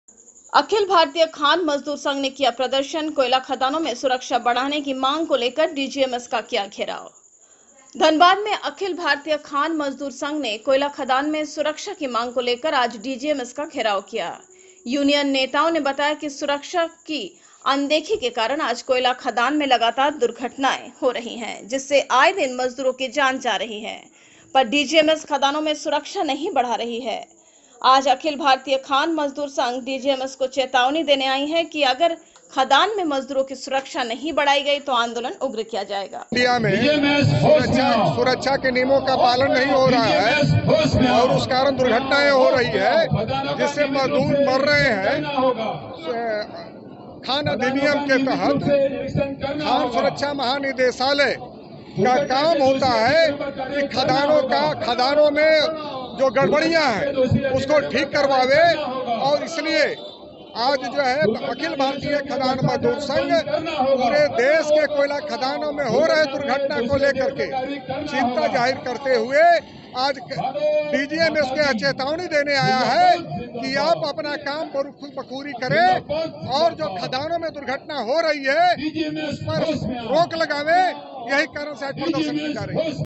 अखिल भारतीय खान मजदूर संघ ने खदानों में सुरक्षा बढ़ाने की मांग को लेकर डीजीएमएस का किया घेराव
अखिल भारतीय खान मजदूर संघ ने किया प्रदर्शन
कोयला खदानों में सुरक्षा बढ़ाने की मांग को लेकर डीजीएमएस का किया  घेराव